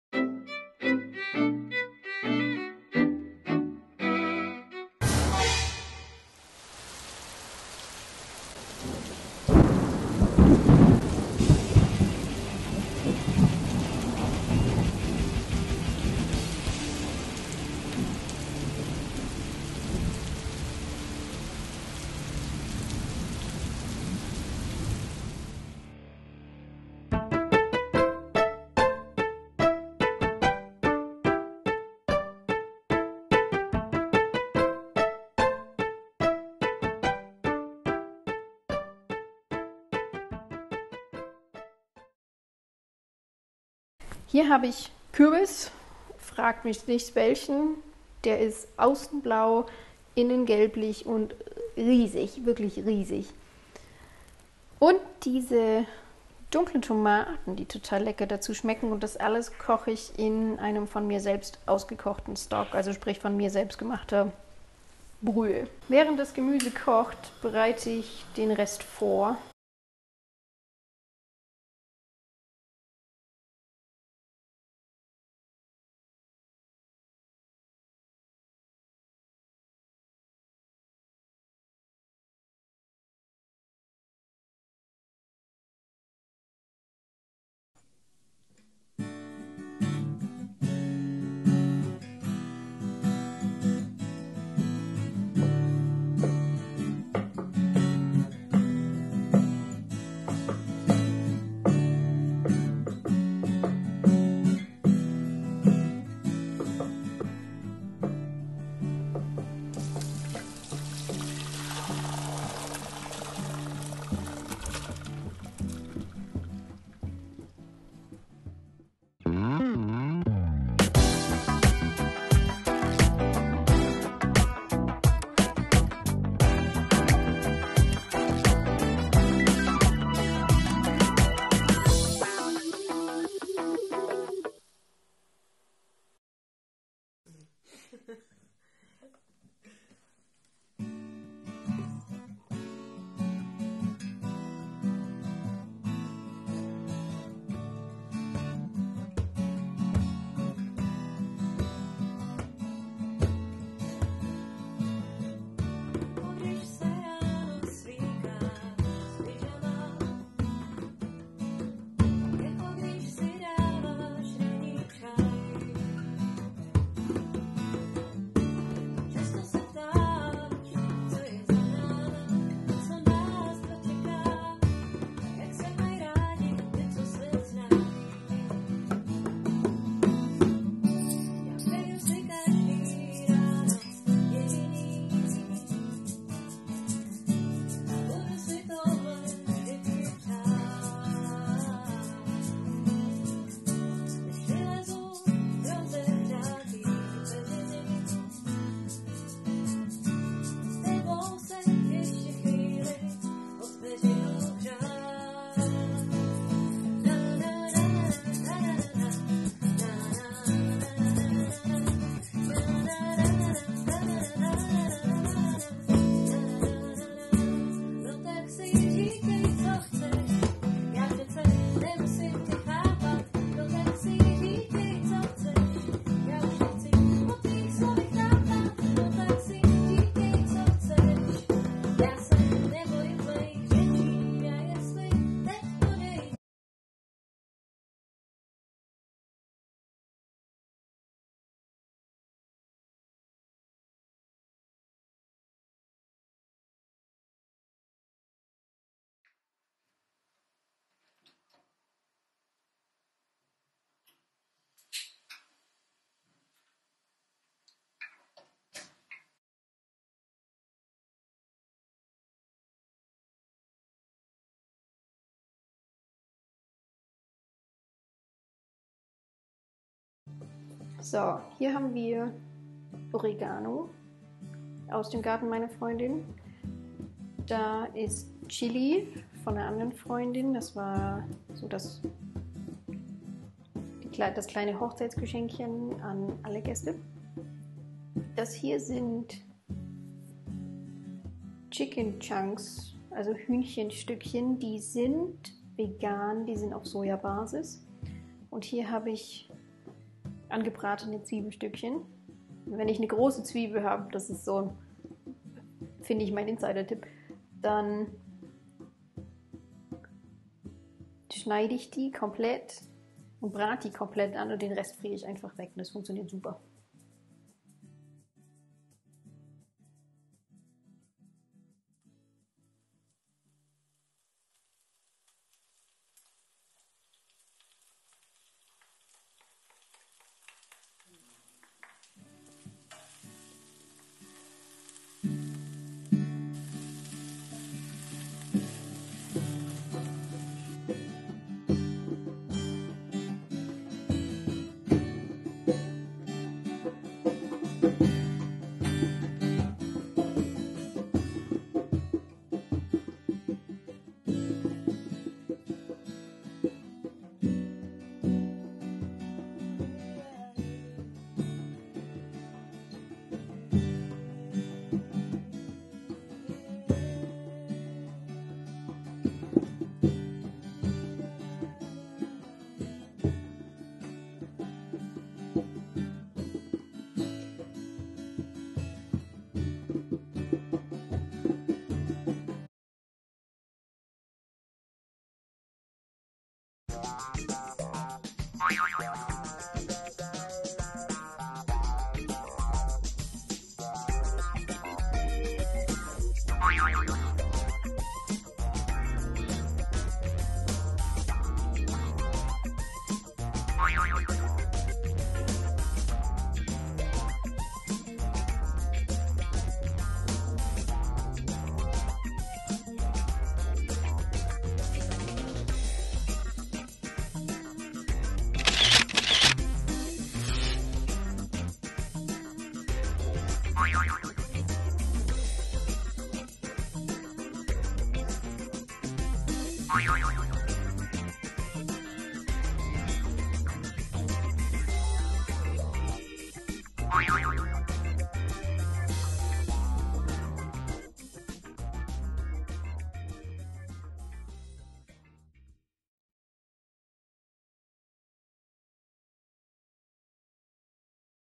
Reispapier Lasagne - vegetarisch! Variante zu Lasagne, leckeres mit Kürbis, Tomate und co.
Man ist das lecker! Und mit veganem Käse gibt es das ganze sogar noch in vegan!

* - Reispapier
* - Kürbis
* - Dunkle Tomate
* - Maioran
* - Oregano
* - Salz
* - Knoblauch eingelegt
* - Ziegenfeta
* - Büffelmozzarella
* - Spargel
* - Basilikumsamen

Das Reispapier nach Packung zubereiten und zwischen die Gemüseschichten legen.

Kürbis und Tomaten vorab 25 Minuten in Brühe dünsten, garen oder kochen lassen, den Spargel ebenfalls vorab verzehrfertig zubereiten.

Mit Käse alles Gemüse und die Gewürze abwechselnd schichten, mit etwas Brühe abschmecken, sodass etwas mehr als der Boden in der Auflaufform mir Flüssigkeit bedeckt ist.
Deko: Basilikumsamen

Ca 20 Min auf 180° Umluft backen e voilá!

Ein Riesen Dankeschön an die Band mačka und ihre zwei bezaubernden Mitglieder für die Musik